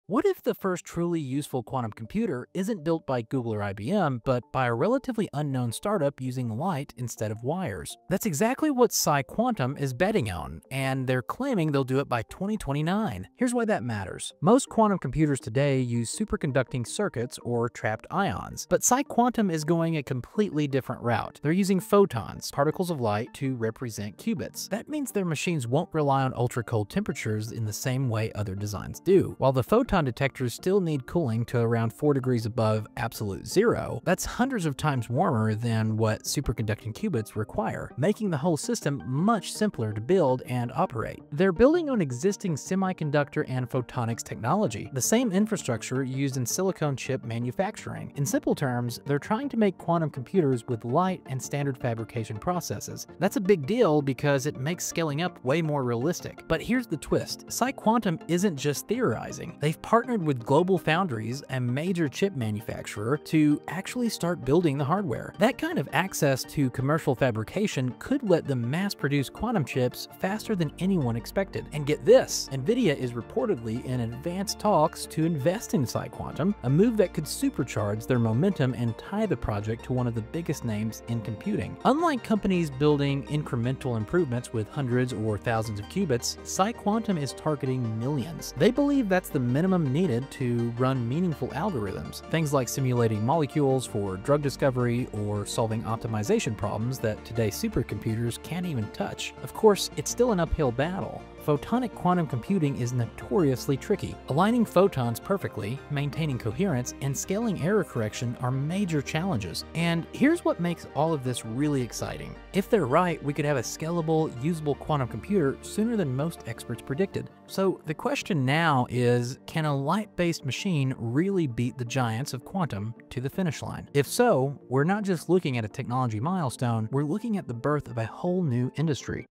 PsiQuantum’s Photonic Quantum Computer: A RACE Against Time and Giants
🔍 Can PsiQuantum beat Google and IBM in the quantum race? Discover how this underdog is using light to build the world’s first useful quantum computer.

PsiQuantum is going all-in on photonic quantum computing—a radically different approach using photons instead of superconducting circuits. In this video, we break down how their light-based technology could leapfrog traditional quantum efforts, why their partnership with GlobalFoundries matters, and what it means for the future of computing.

🔗 Topics covered:
– What makes photonic quantum computing different
– Why PsiQuantum’s 2029 goal is bold but possible
– The importance of quantum error correction
– Nvidia’s rumored investment in PsiQuantum
– The real-world implications for science, AI, and industry